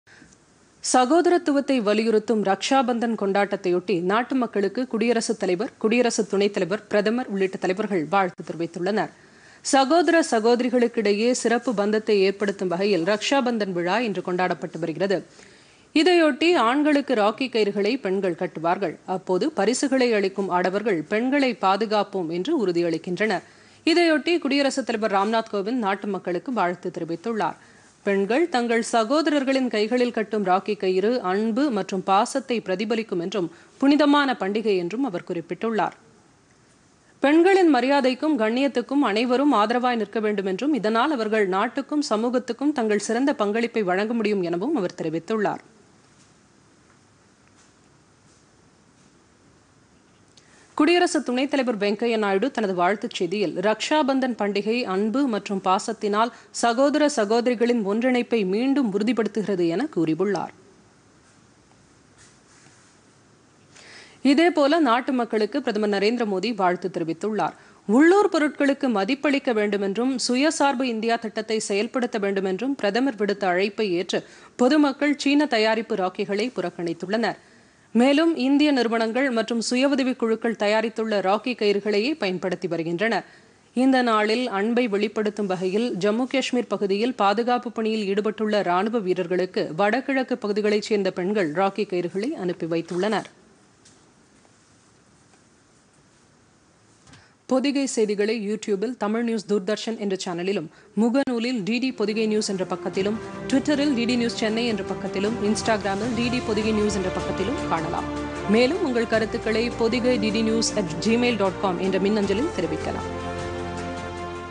ரக்ஷா பந்தன் - தலைவர்கள் வாழ்த்து
ரக்ஷா பந்தன் விழாவையொட்டி, நாட்டு மக்களுக்கு குடியரசுத் தலைவர், பிரதமர் உள்ளிட்ட தலைவர்கள் வாழ்த்து - உள்ளூர் தயாரிப்பு ராக்கிகளையே பயன்படுத்தும் மக்கள்.